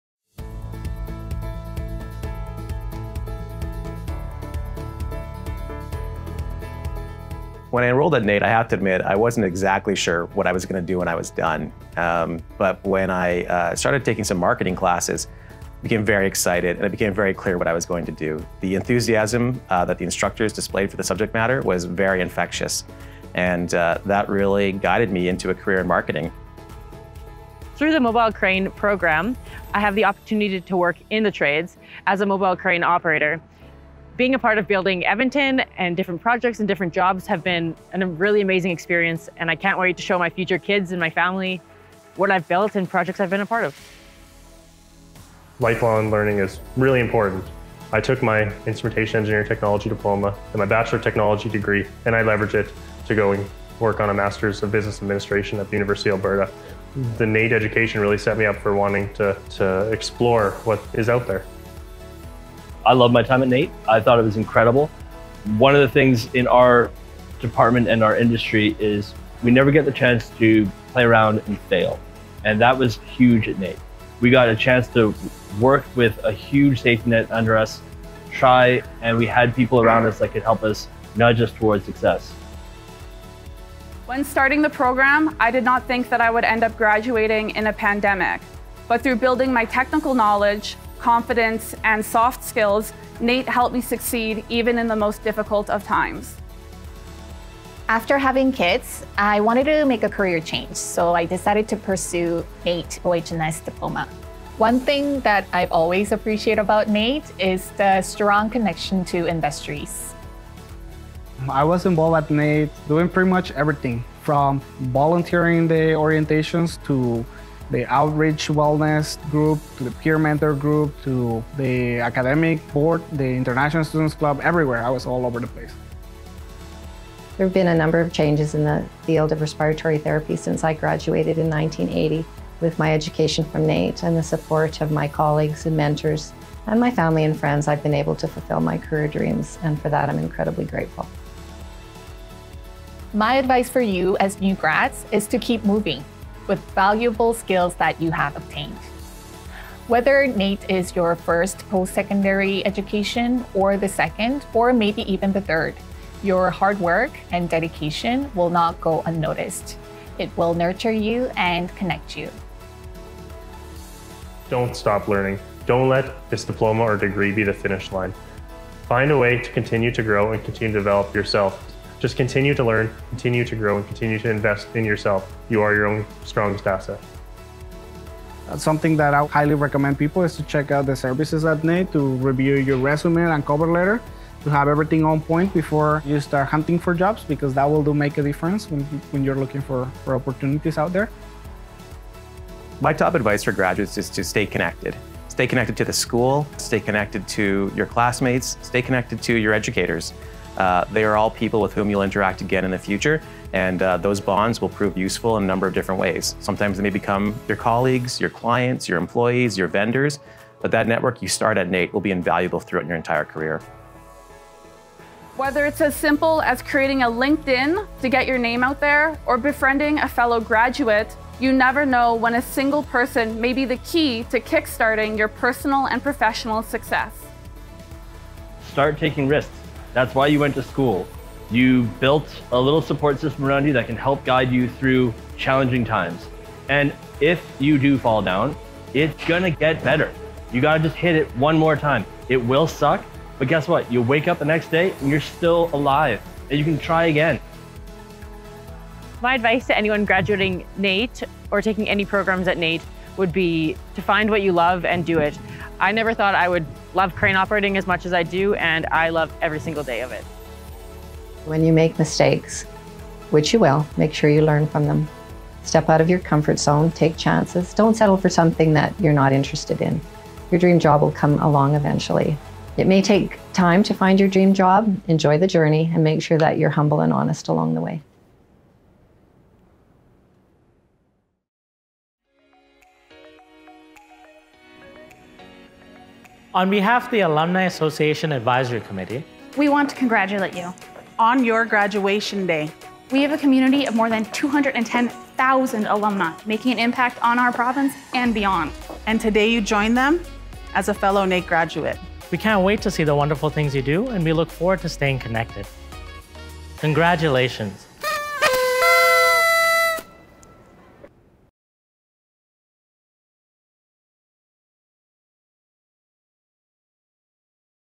Convocation 2022 - A message from NAIT Alumni
An inspiring message for new graduates from NAIT alumni.